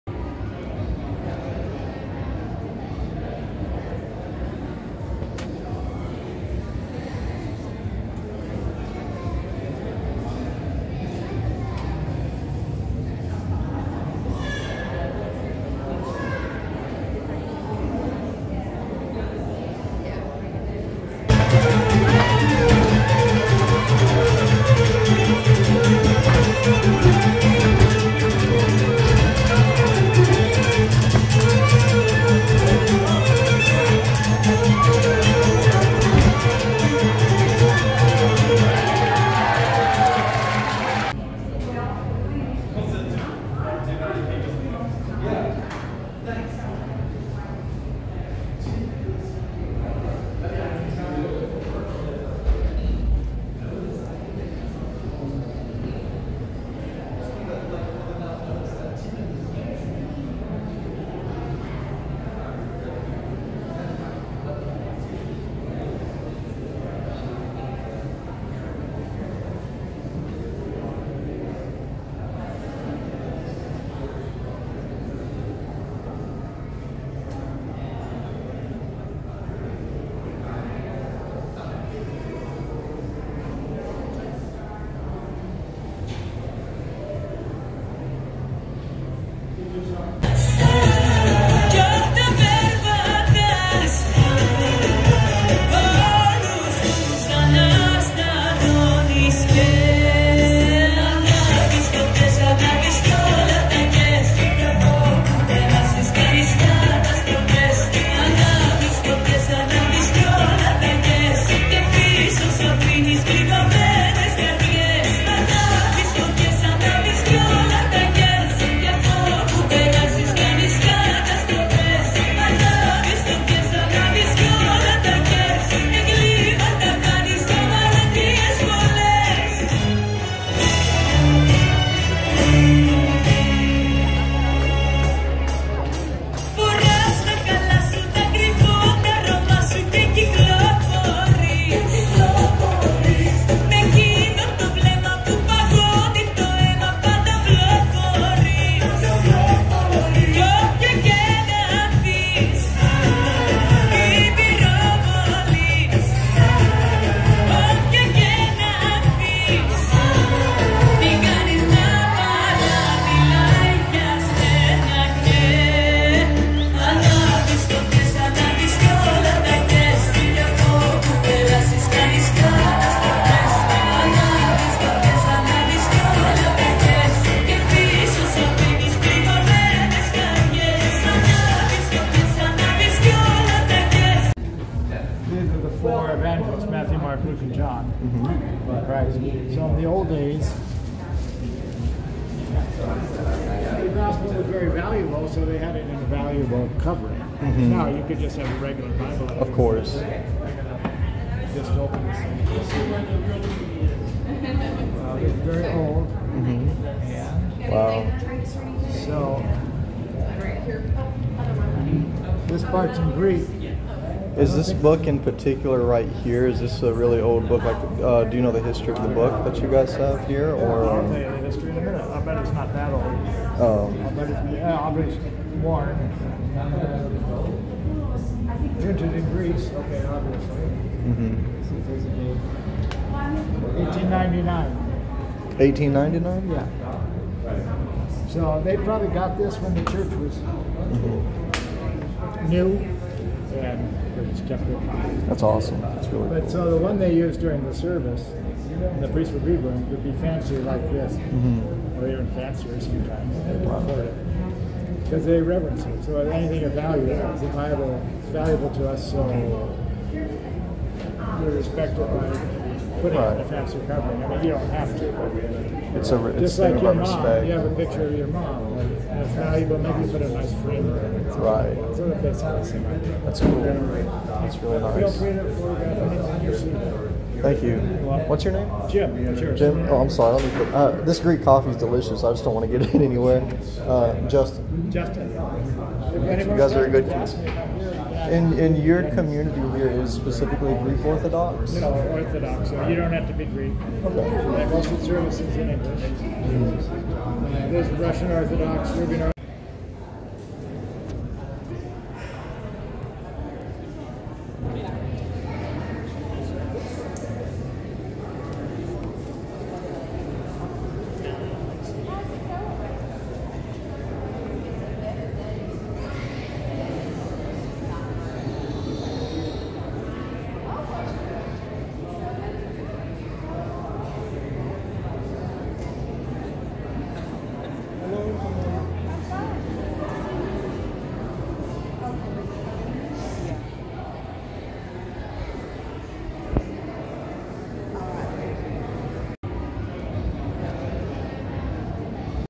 Beautiful Greek Orthodox Church And Fun Greek Dancers!
Footage of the 50th annual Greek festival held in Birmingham Alabama.